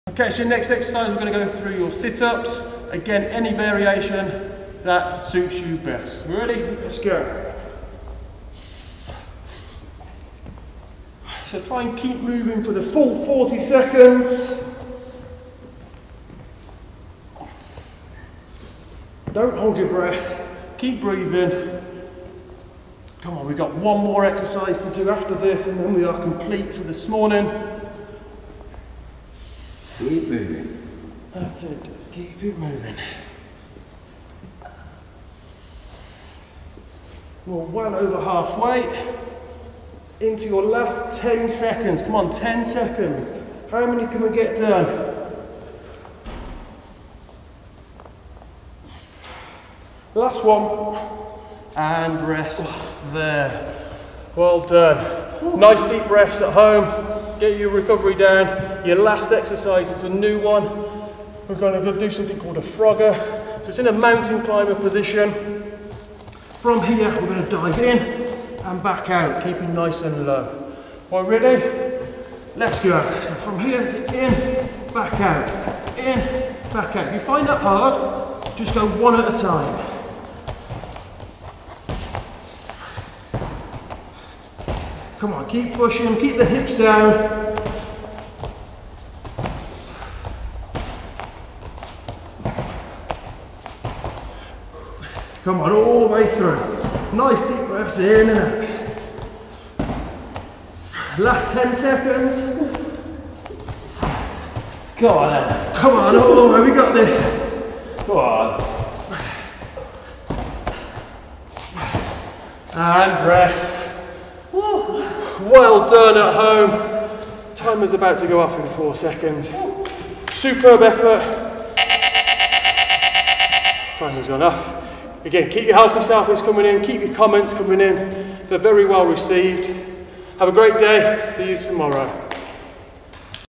Boot camp 2, part 2
Daily exercise video with our Mr Gallinagh and other members of St Bede's staff - join in and post pics with the getupwithgallers hashtag!